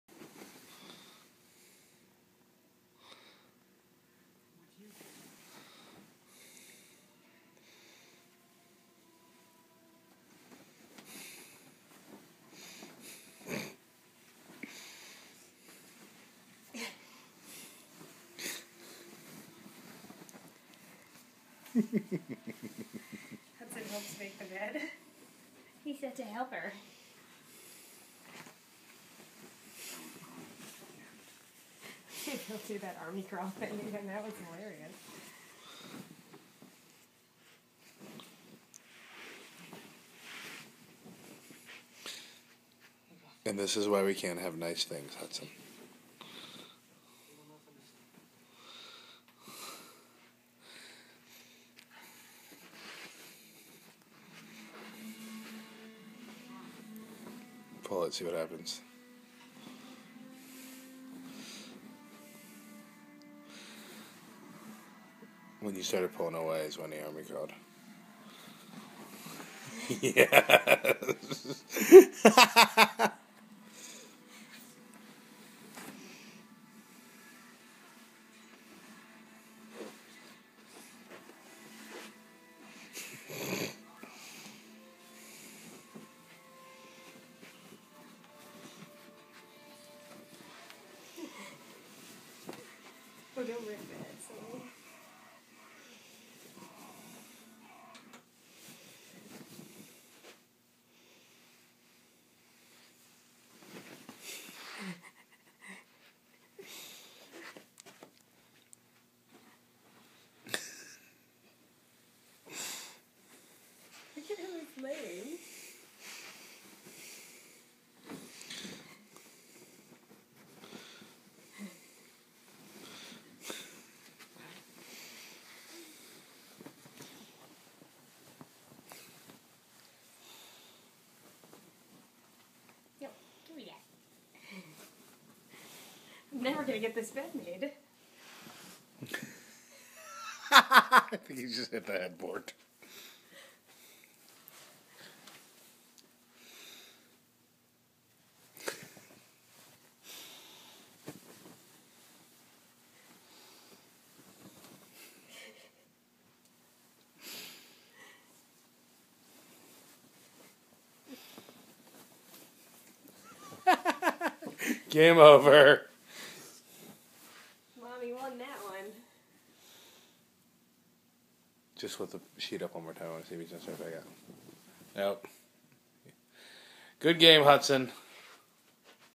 Hudson hates bed sheets
Our cat does not like it when we make the bed